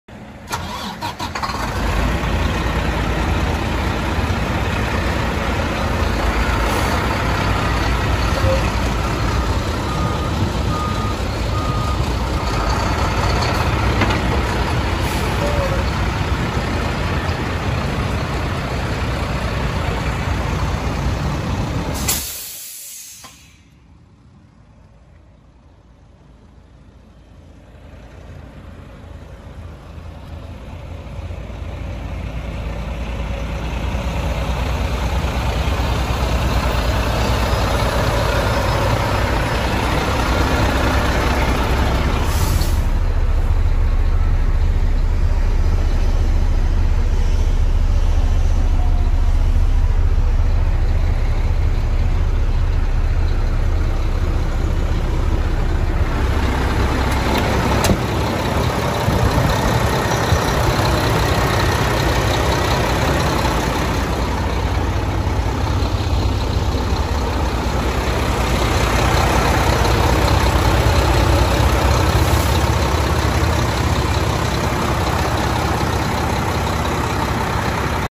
2012 MACK GRANITE GU813 For Sale
Thats Right Only 9302 Miles!!, Exhaust brake, Allison 4000 RDS, Diff lock, Double Frame, Block Heater, Both seats air ride, aluminum tank, power rt window, 25 foot of double frame,